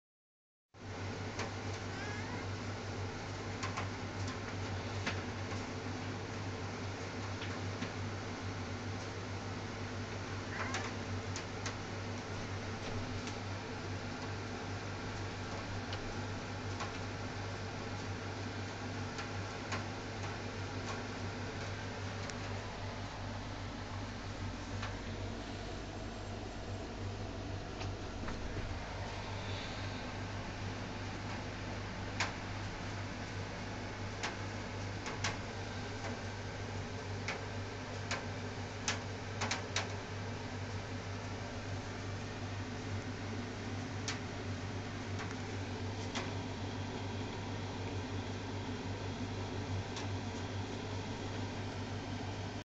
Lupin meows for toy
Lupin is a blue-mitted ragdoll. Born on 22Nov 2011.

He likes play with my hairband :P